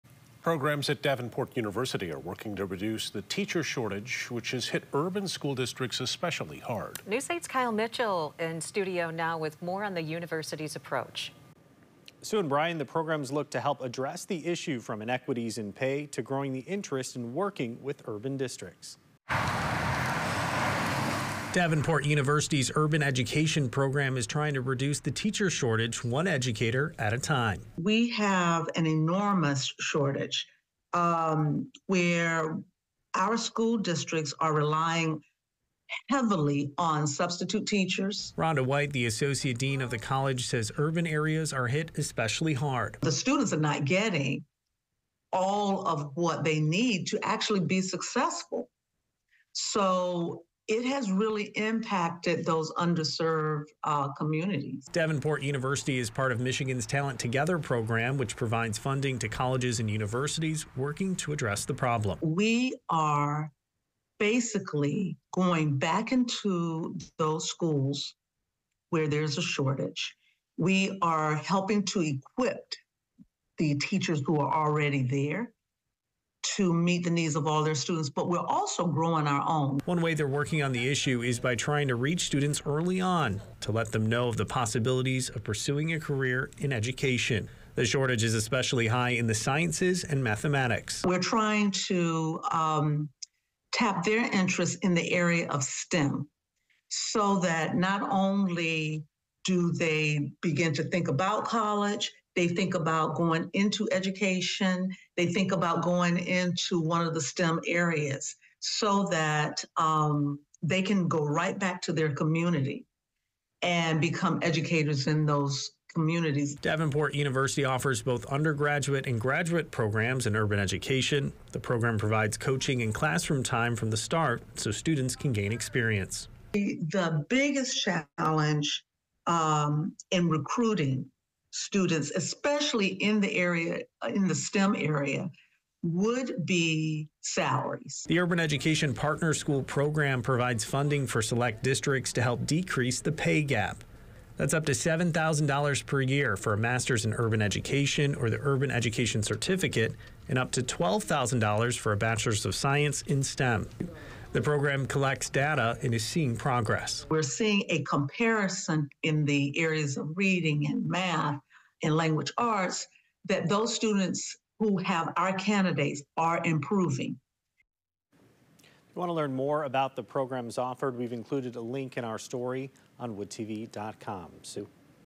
How Davenport University is working to address urban teacher shortage
Programs at Davenport University are working to get more teachers into urban school districts. (Jan. 29, 2024)